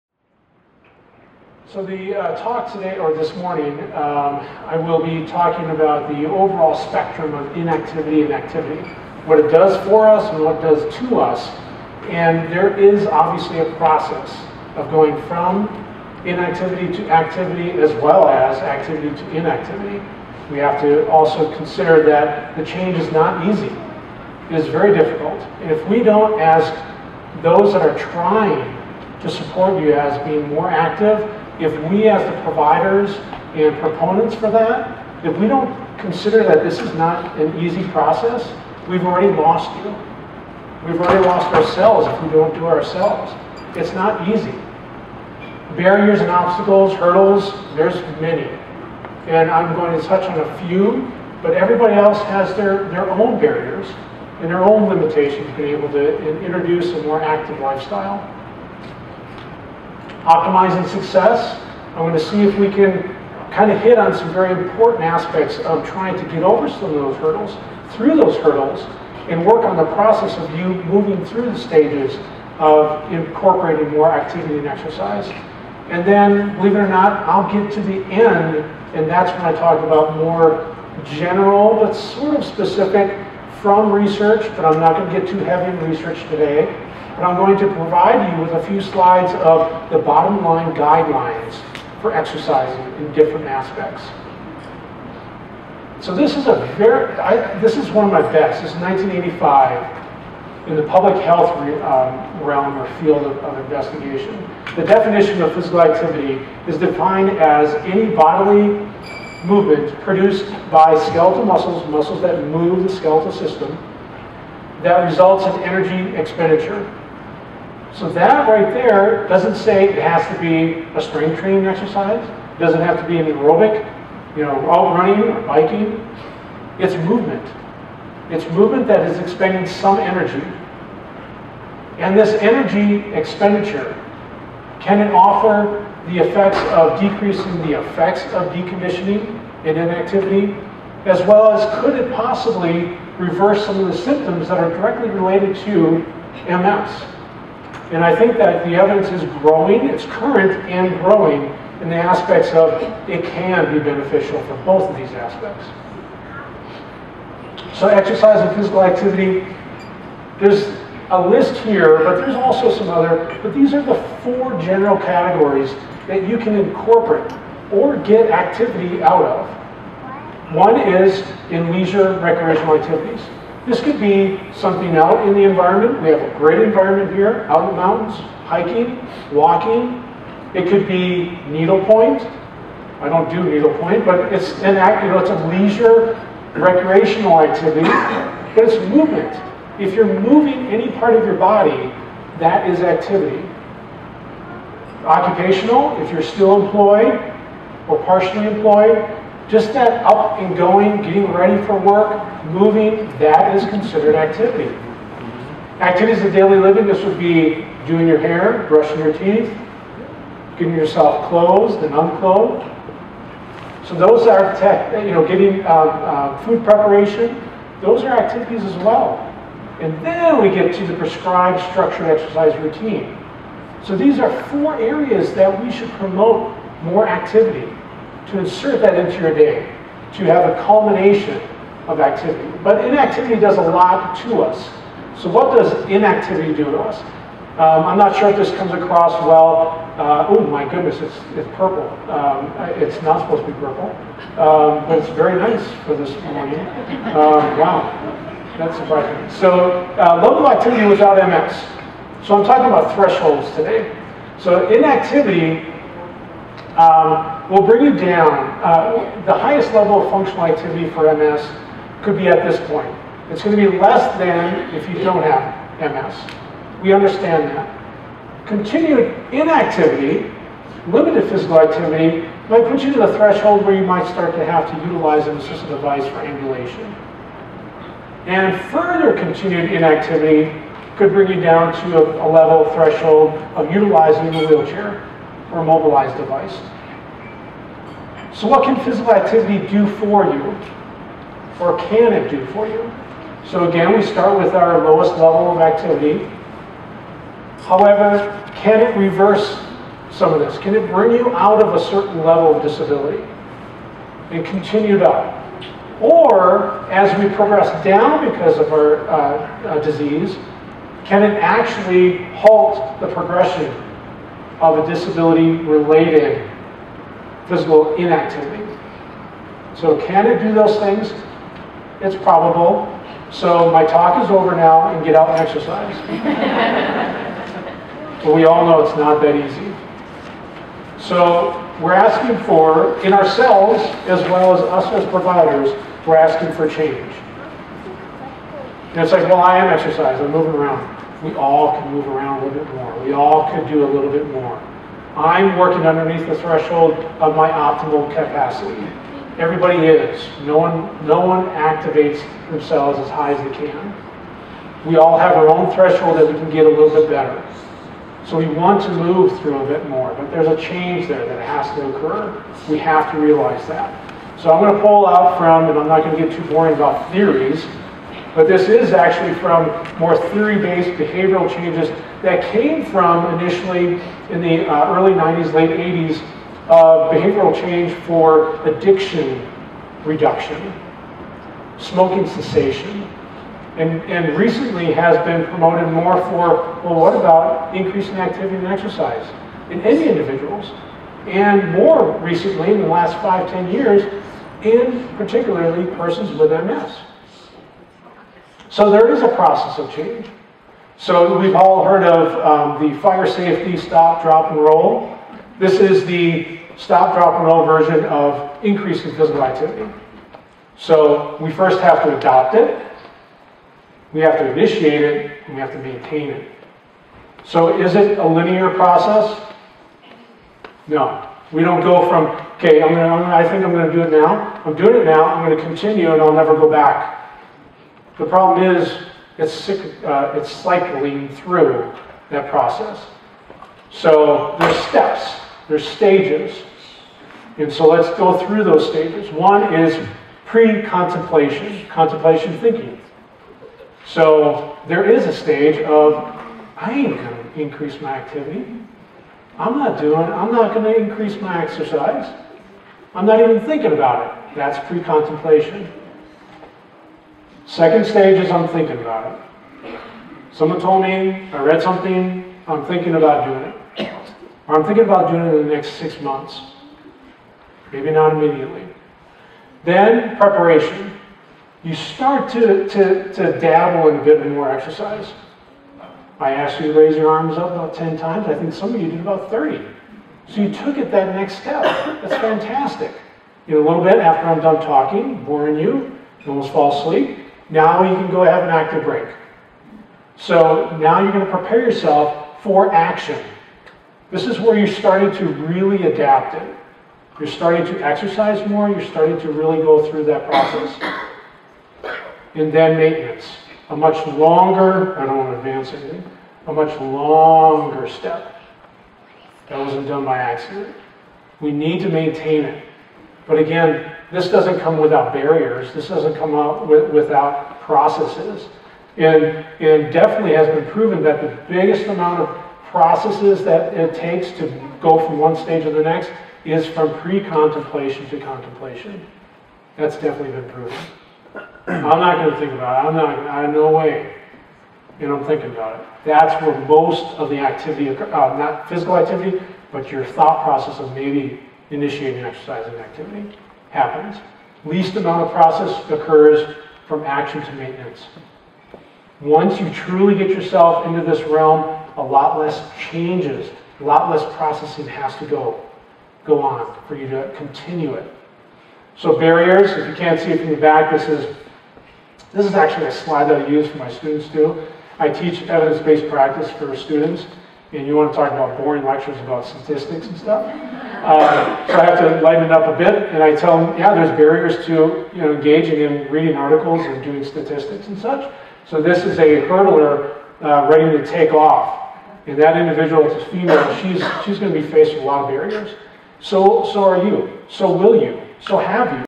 Exercise & Multiple Sclerosis Part 1
Jeff Hebert, PhD, explores the impact of exercise on managing multiple sclerosis